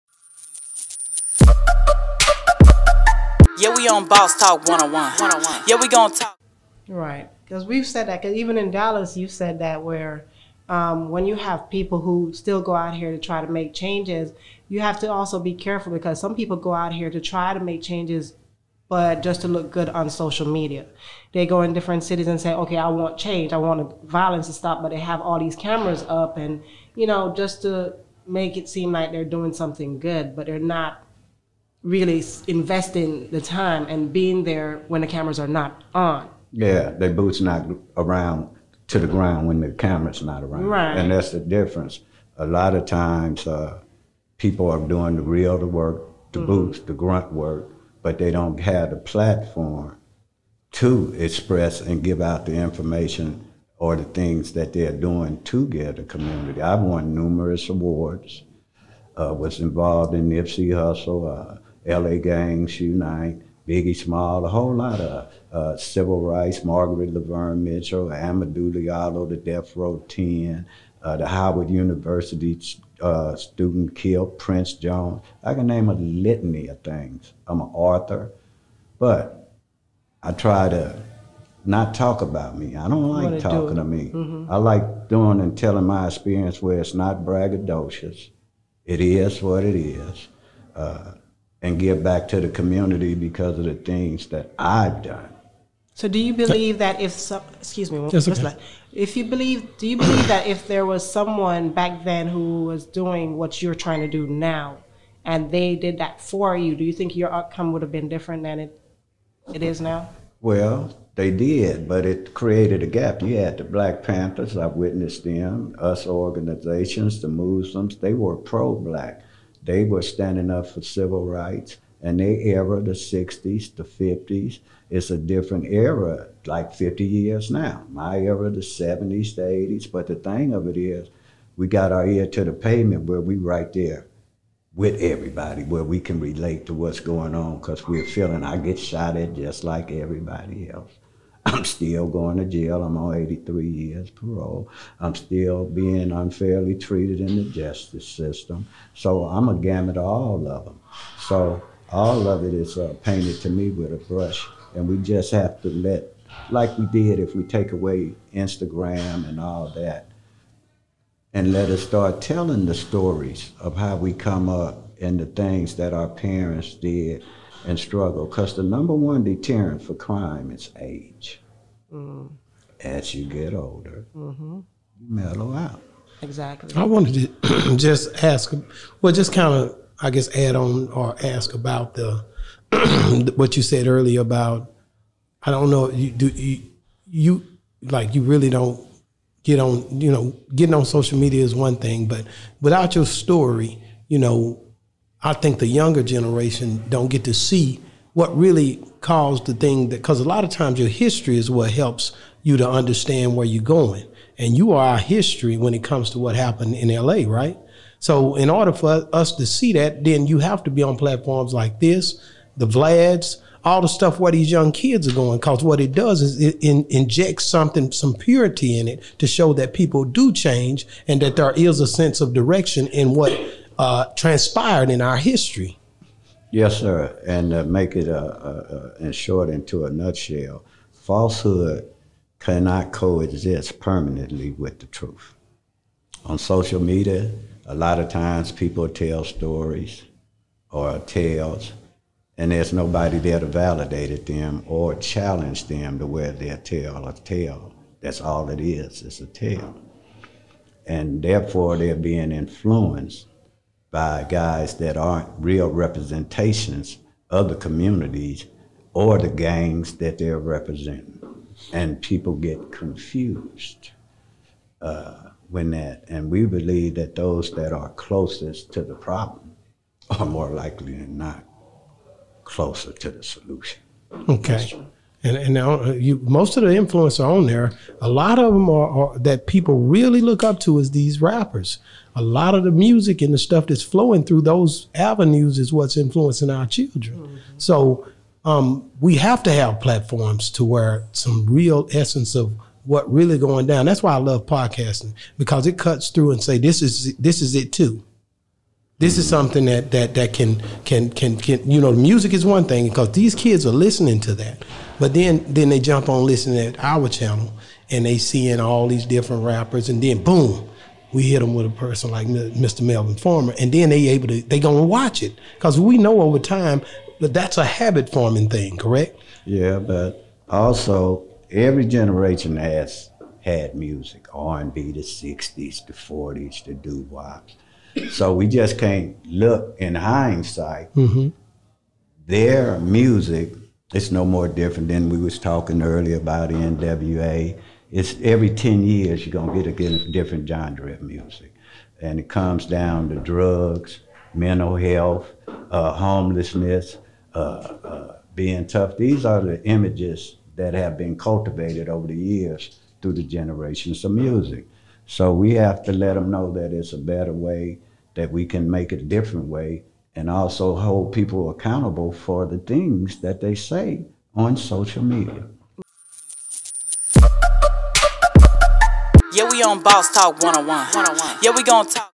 Melvin Farmer (Part 4) Nipsey Hussle Biggie Smalls Shootings I get shot at like everybody else +More
Melvin Farmer we have to Inject Truth Social Media's Misleading Im still in the streets close to the problem trying to find resolution I was awarded for getting involved with the Issues in the streets to help after the situation happen